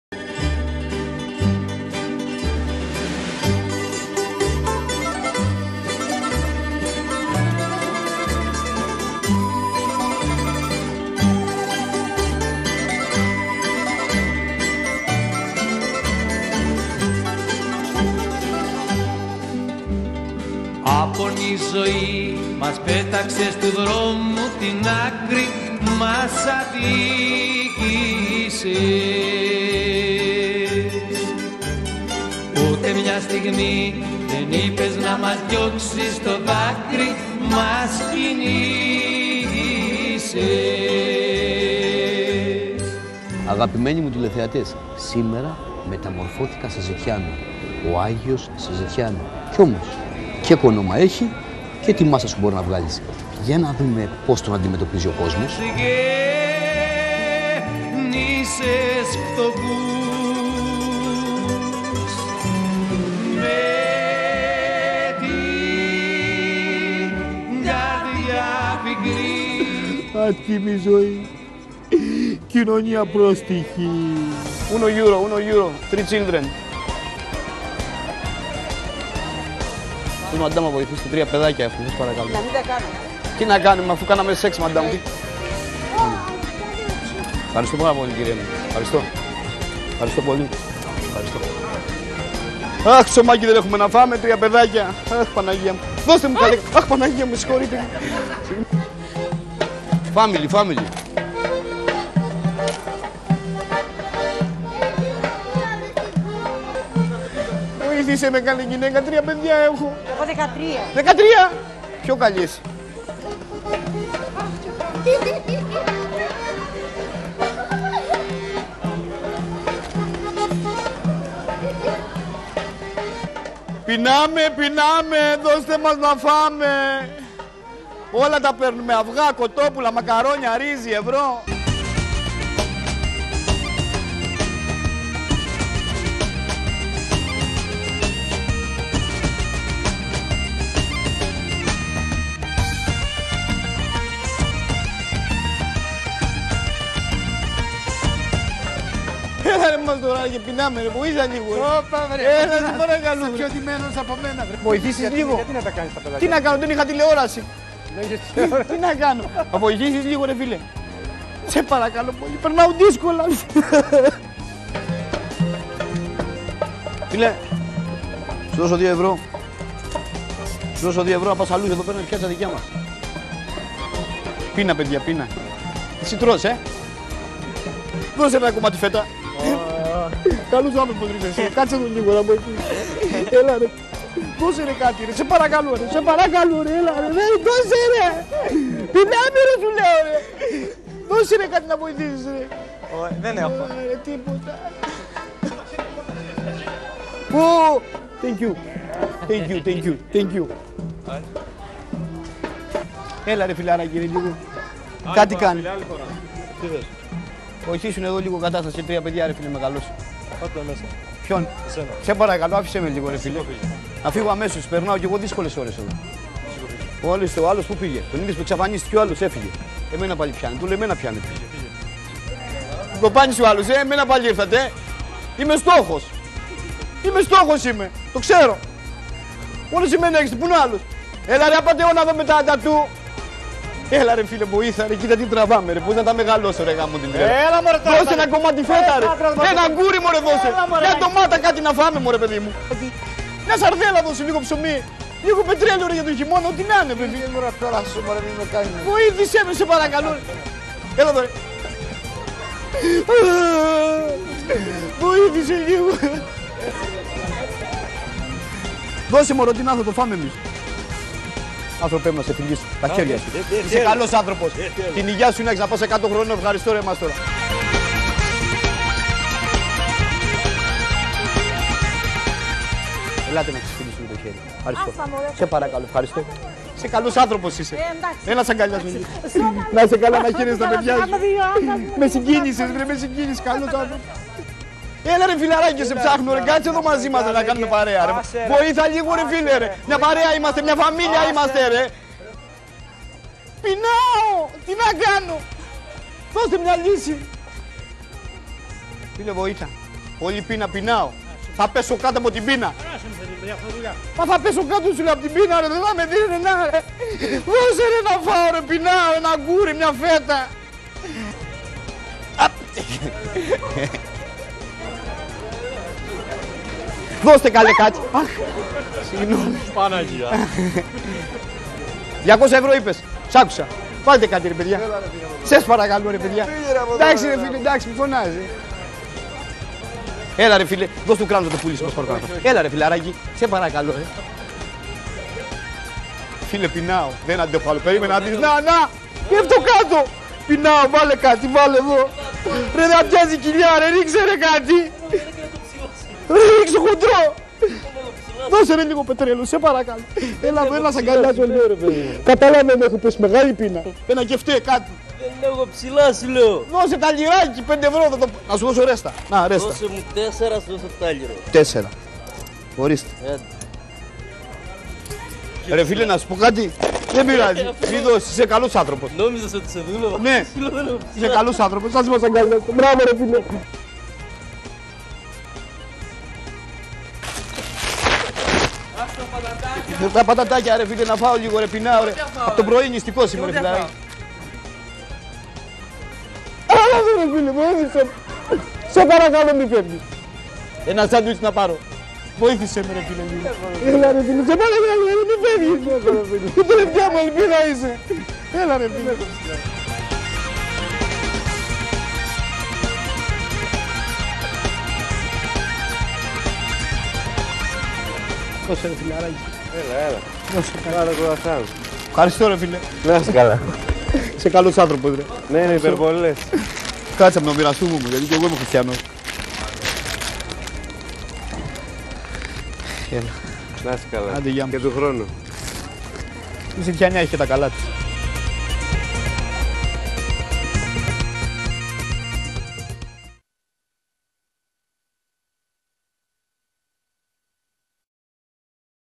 Ο Άγιος μεταμφιεσμένος σε ζητιάνο
Η σελίδα στο Facebook - Στο κεντρικό δρόμο του Ηρακλείου στη Δαιδάλου, ο Άγιος στήνει ένα τρελό πανηγύρι κάνοντας το ζητιάνο. Απίστευτο!